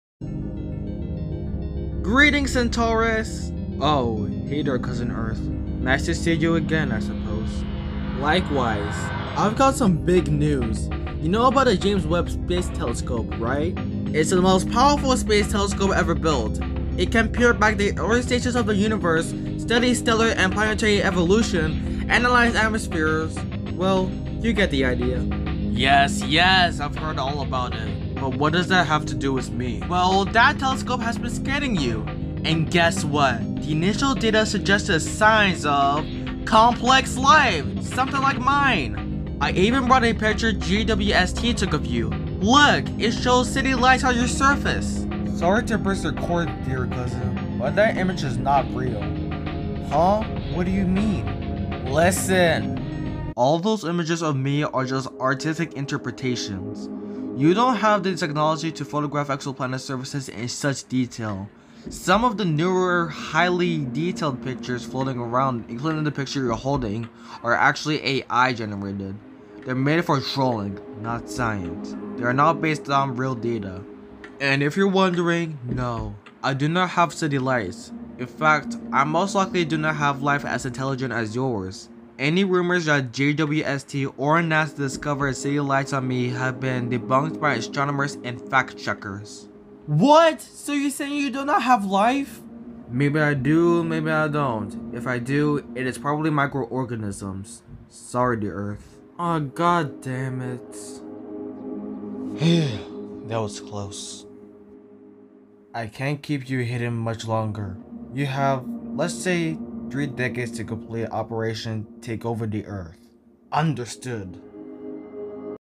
Is There City Lights on Proxima Centauri b?
The earth excitedly shows Proxima Centauri b an image supposedly taken by the James Webb Space Telescope (JWST) showing city lights on its surface. However, Proxima Centauri b explains that this image, and others like it, are not real.